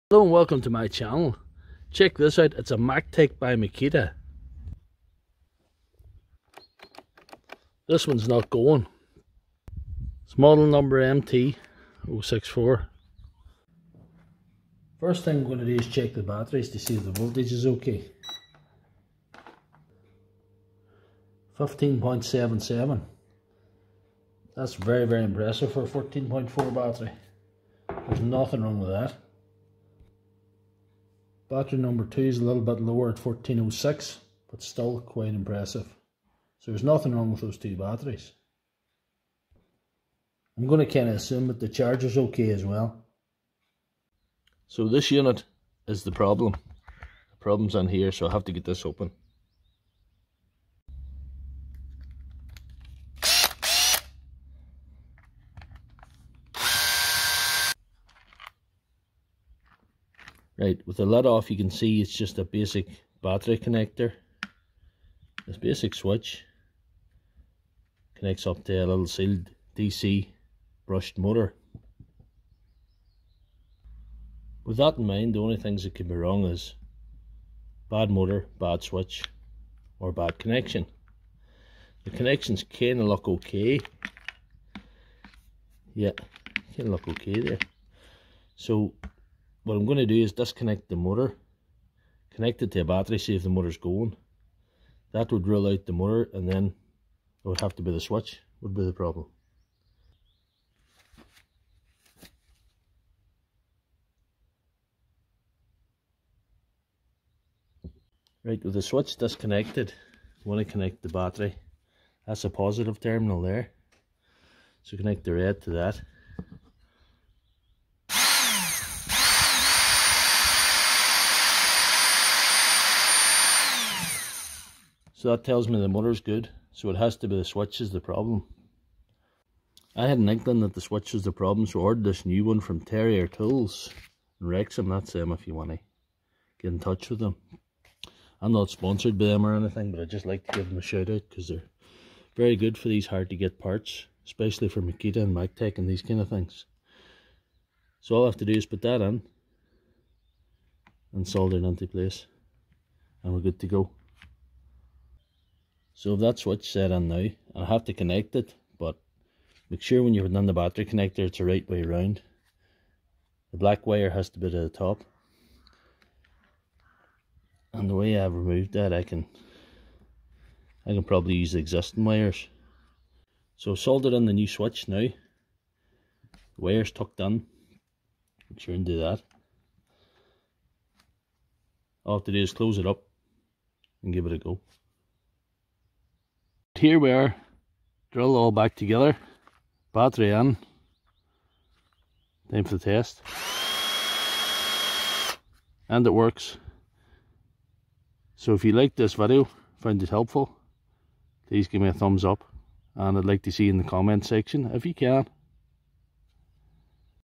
Makita Maktec not going? Try This!!powertools
I was given this maktec 14.4v ni cad drill to repair. It is a Maktec MT064 14.4v nicad drill which is old technology Now. Makita make these Maktec machines and they are the budget version of some of their main line of tools. They are distinguished from Makita by the colour and the name. The drill featured here has an issue it does not work upon testing the batteries the main culprit it ni cad machines I found both to be in good condition this quickly ruled them out as an issue by testing the voltage with my Venlab Digital Multimeter VM-600A. I moved on to the drill assuming the charger was good. I opened the drill to find a basic setup. It had a sealed DC motor a switch going to battery connector. I quickly established the motor was OK by disconnecting it from the switch and connecting the battery directly to it. That meant the switch was the issue. I had already taken the liberty of ordering the switch for it because I had a guess that it was the issue. I got the switch from Terrier Tools in Wrexham who are a great source of spares for anyone UK based. I quickly soldered in the switch using my M12SI Milwaukee soldering iron put it together and it is back in running order.
This video will show you simple ways to test your drill to find the fault.
items used in the repair below
Milwaukee M12 Soldering iron